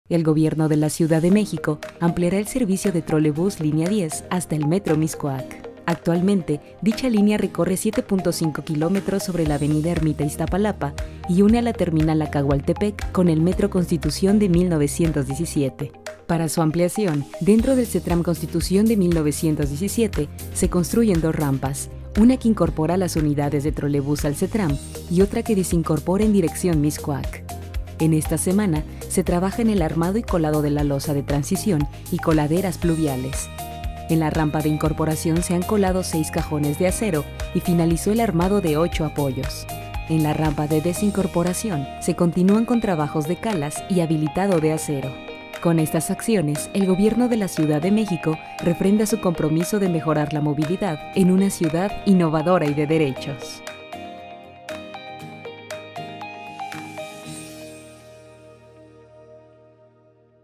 ¡CONTINUAMOS TRABAJANDO EN LAS GAZAS DE INCORPORACIÓN Y DESINCORPORACIÓN DEL TROLEBÚS ELEVADO.🚎
¡Continuamos trabajando en las gazas de Incorporación y Desincorporación del Servicio de Transportes Eléctricos de la Ciudad de México.🚎

Estas gazas ubicadas en el CETRAM de Constitución 1917, permitirán la expansión del servicio en la Línea 10 del Trolebús hasta el metro Mixcoac.

Conoce los avances de esta semana.🚧